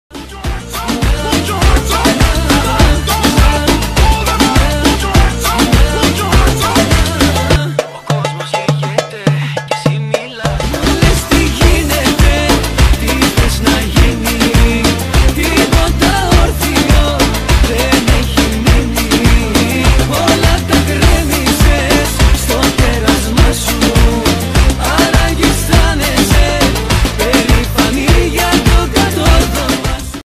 PANOS KALIDIS at Chameleon Club Dubai
What a great night at Chameleon Club Dubai with the Greek star "PANOS KALIDIS - Πάνος Καλίδης"!